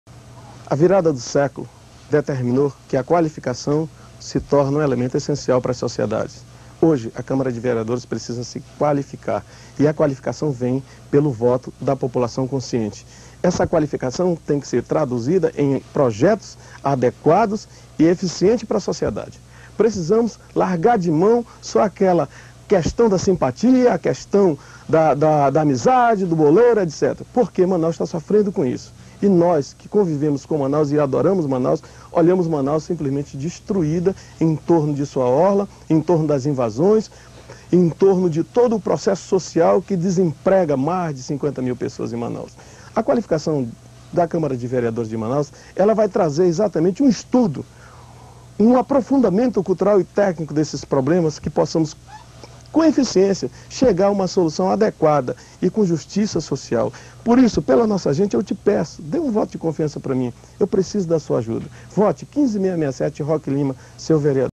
Roque Lima PMDB - Candidato a vereador
Roque Lima, candidato a vereador na cidade de Manaus, pelo PMDB, nas eleições de 2000. Fala da qualificação da câmara.

NOS ACOMPANHE!

Rua Professor Samuel Benchimol, 477 - Parque 10 de Novembro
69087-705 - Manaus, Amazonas